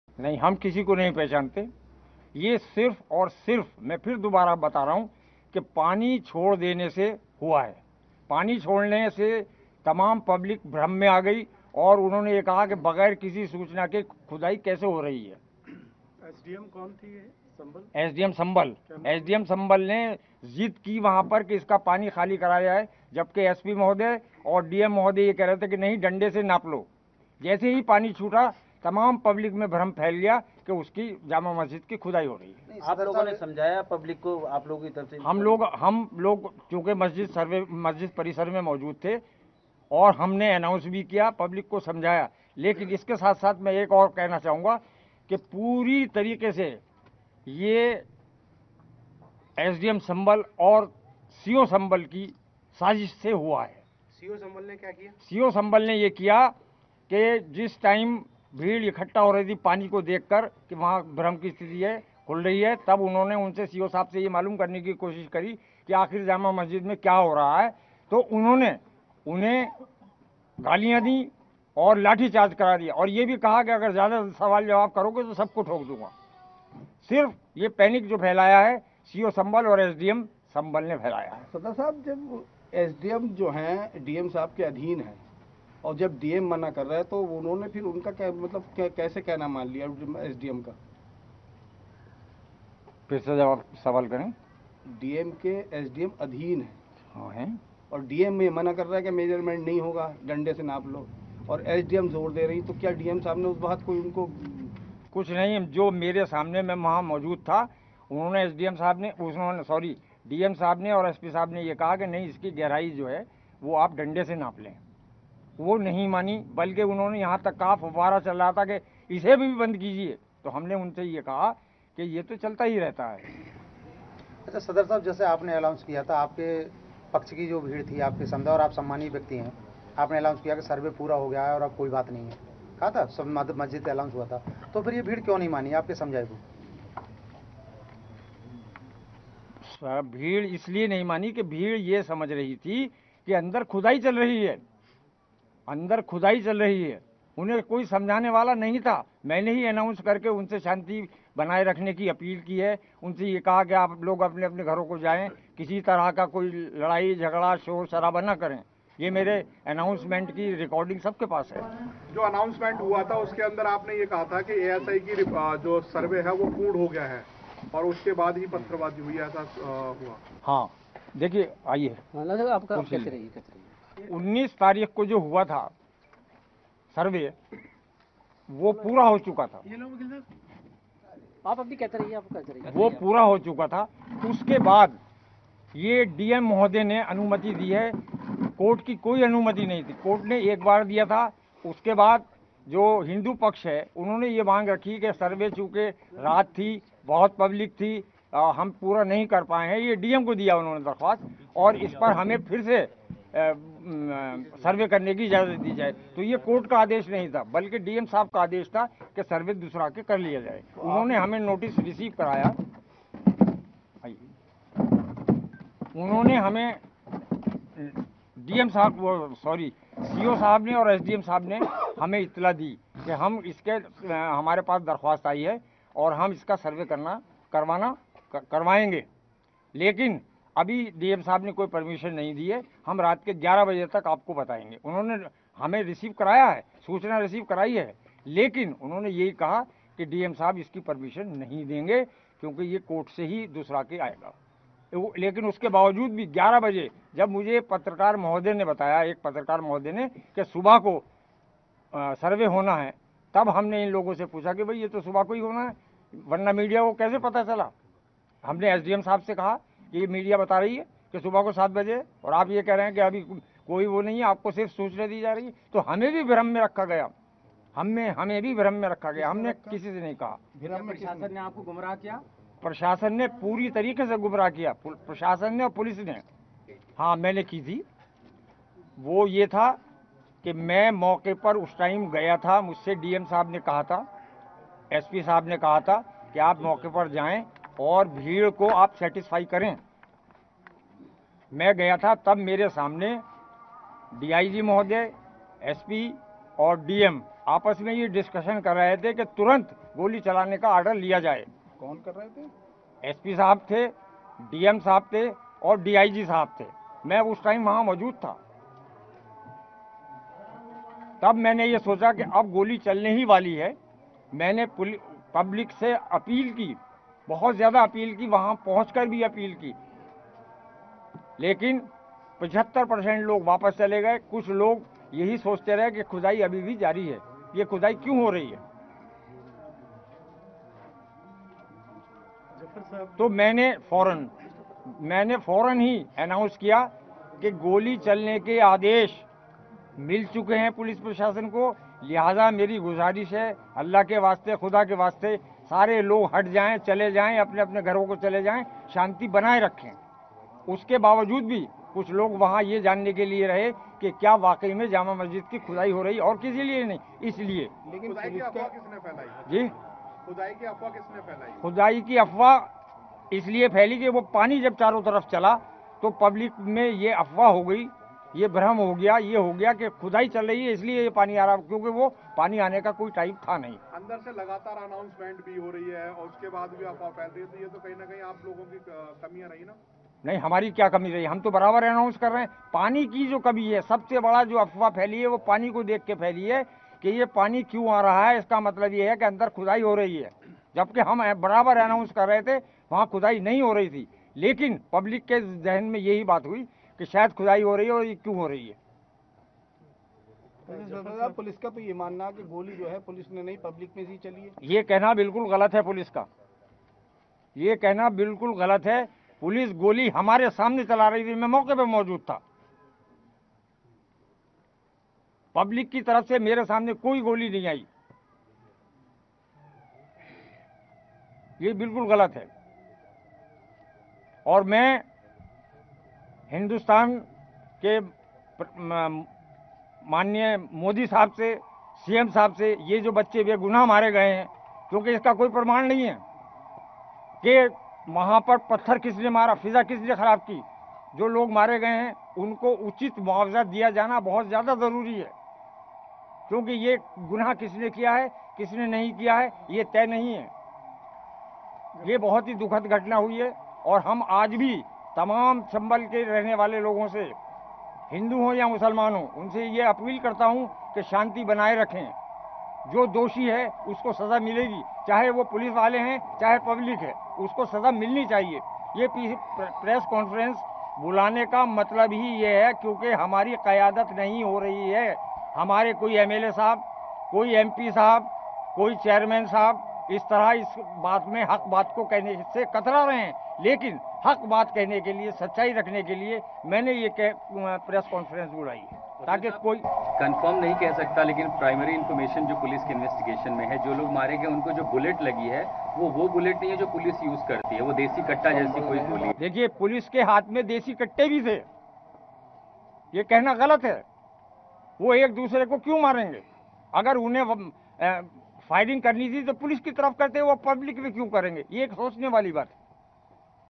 Sambhal Violence News | Mosque Sadar Chief Zafar Ali ने PM Modi से की ये अपील! | News18 Haryana
In the wake of brewing violence in Uttar Pradesh’s Sambhal, Mosque Sadar Chief Zafar Ali made this appeal to Prime Minister Narendra Modi and state Chief Minister Yogi Adityanath on November 25. Watch the Video!